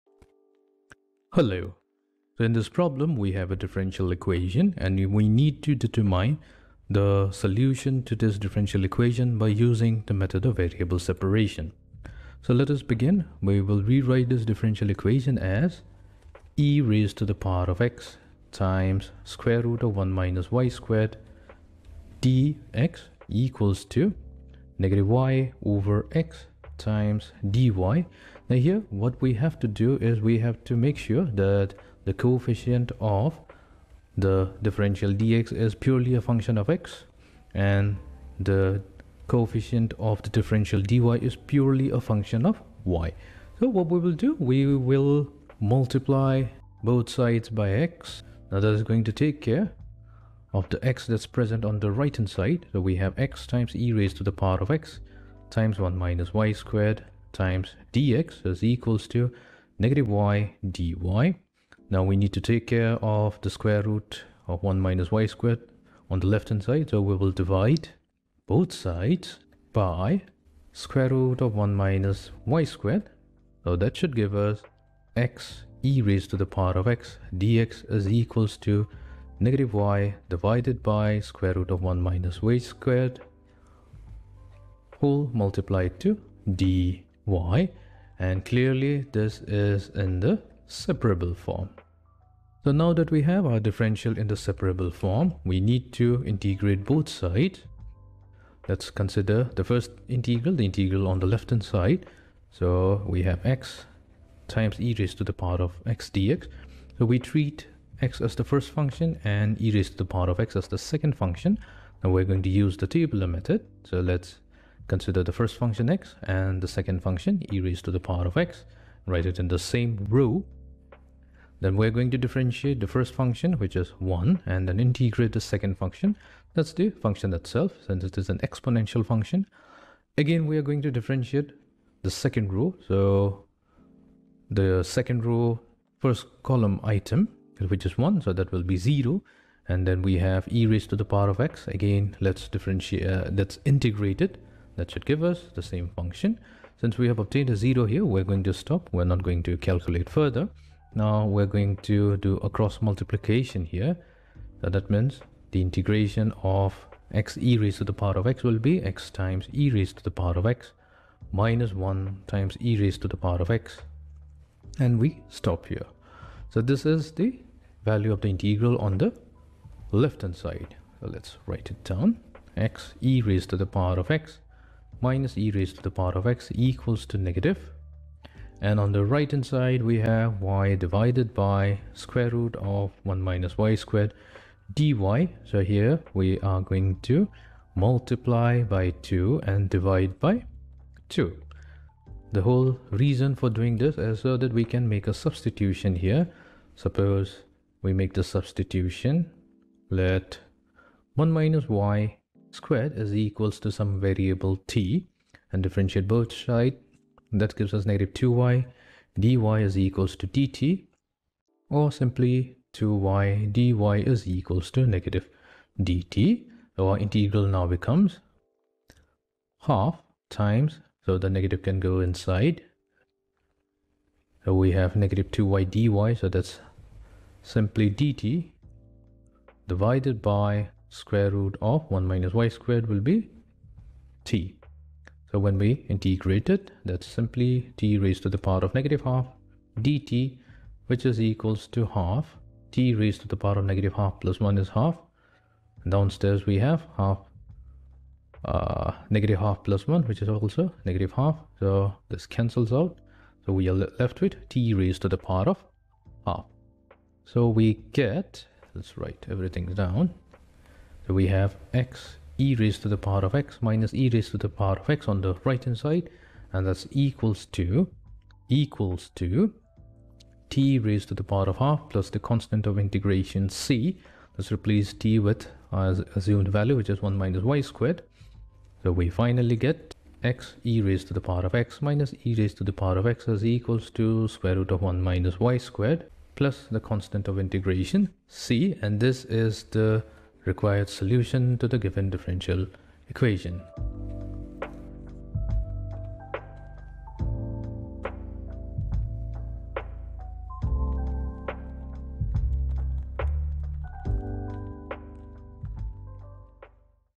10.3 - Differential Equations | Method of Variable Separation - TYPE III
10.3 - Differential Equations | Method of Variable Separation - TYPE III

In this problem walk-through video, we'll be looking at a first-order linear differential equation and solving it using the variable separation approach. To begin, we'll write dx and dy as differential equations to isolate the variable y on the right side. The solution to the differential equation will then be determined using some integration techniques.

About